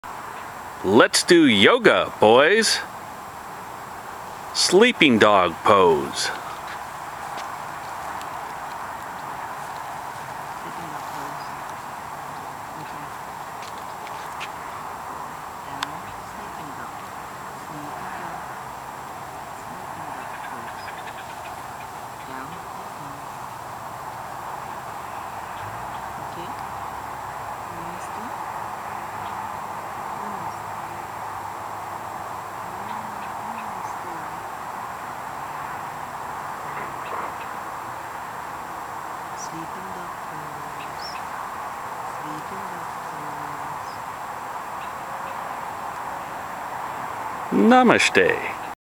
Basset Yoga
Two basset hounds during their yoga routine...